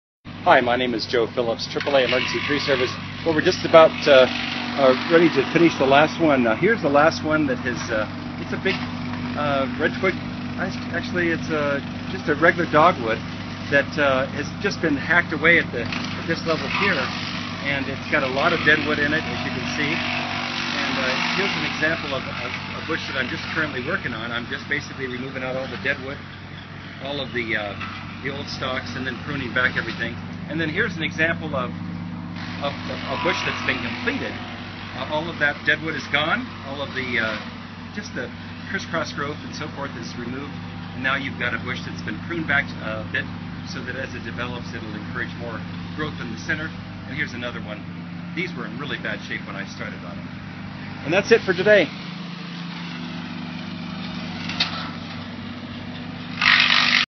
Denver Dogwood Deadwood Trimming
This video shows the work done for a Dogwood and Deadwood trimming in Denver. Removed all

dead and damaged growth.

Corporate Headquarters Address:
AAA Emergency Tree Service, LLC
1115 West 10th Avenue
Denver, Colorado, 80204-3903